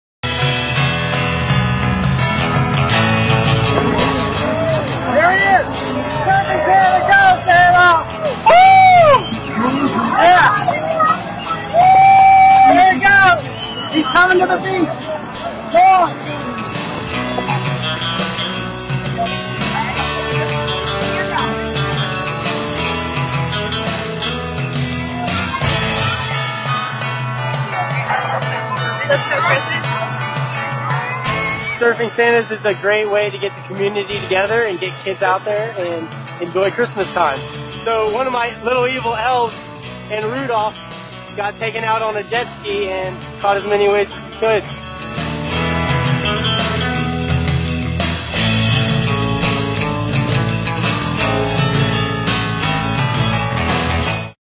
Surfing Santa Arrives in Cocoa Beach - Surfing Santas of Cocoa Beach
Amazing media coverage of our event. Thank you everyone for all the support!

Text SurfingSantas to 41411 for real time event updates!